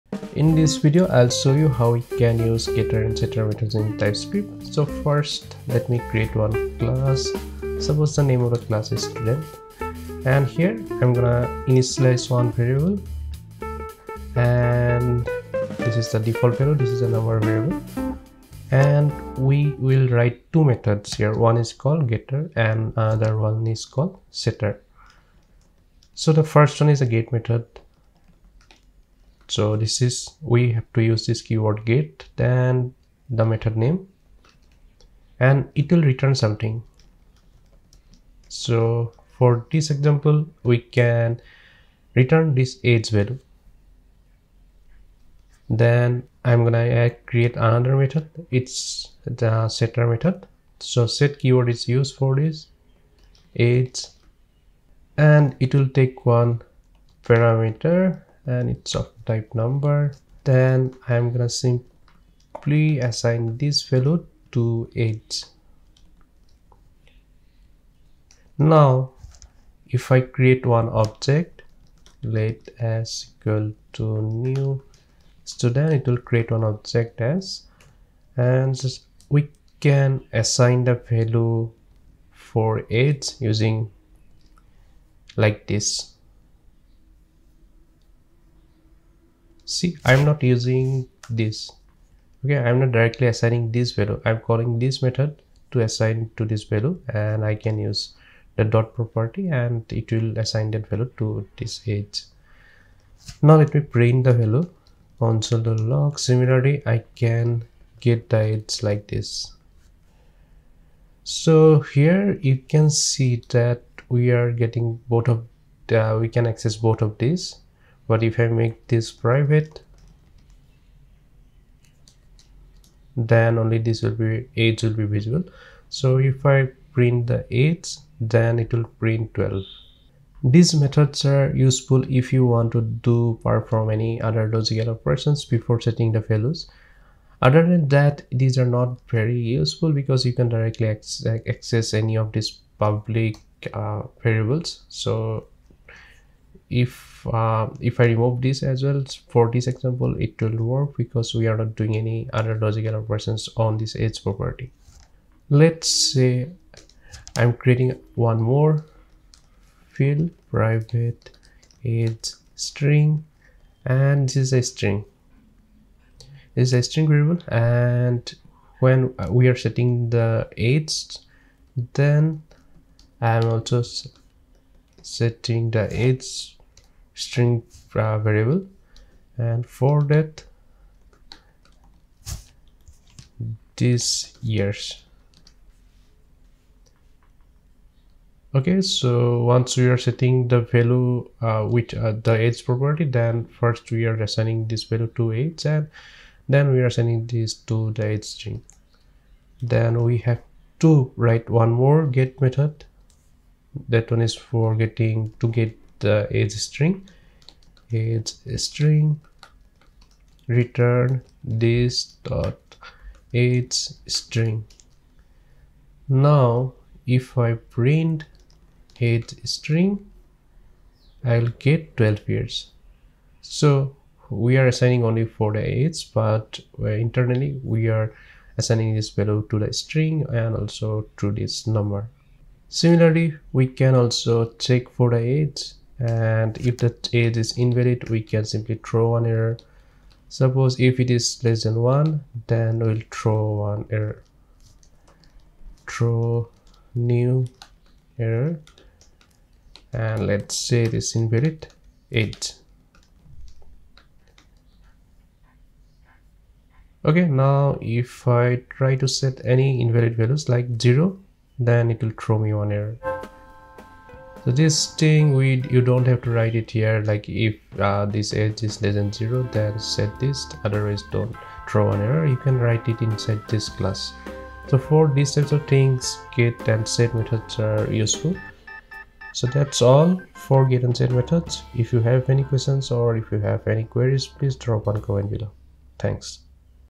TypeScript tutorial 13: TypeScript Getter and Setter | How to use Getter/Setter in TypeScript
How to use Getter/Setter in TypeScript.
In this video, I am showing how and why to use Getter/Setter methods in TypeScript with examples.